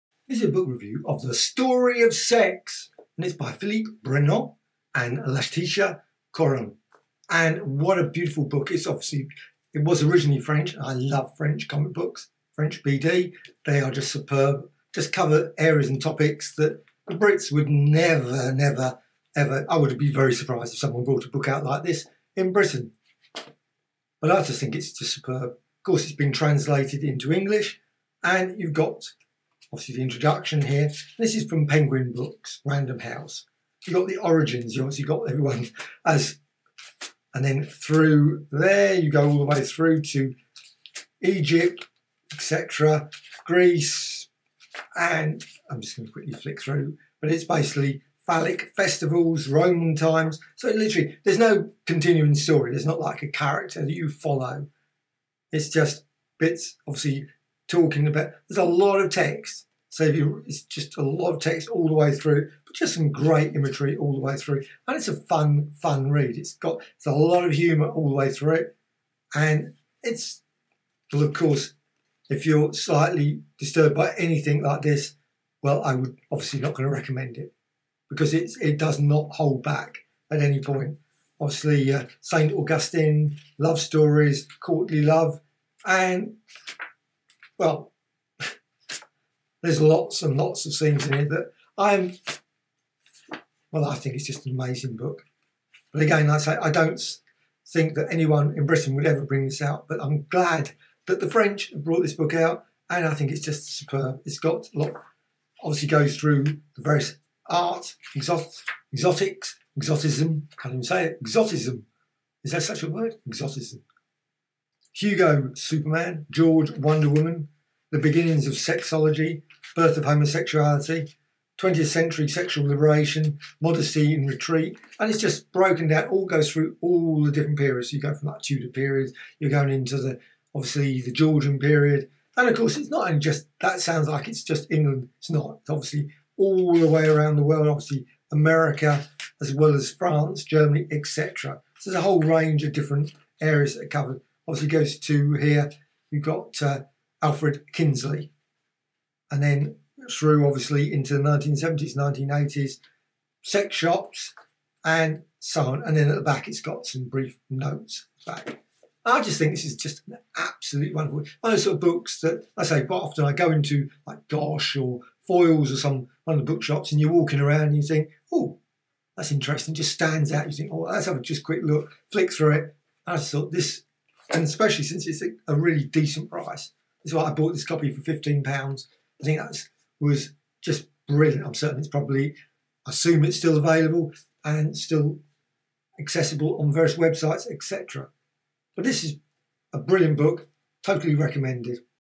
The Story of Sex by Philippe Brenot and Laetitia Coryn Book Review
The Story of Sex by Philippe Brenot and Laetitia Coryn Book Review - Tudor, Roman, Egyptian etc - superb graphic novel / historical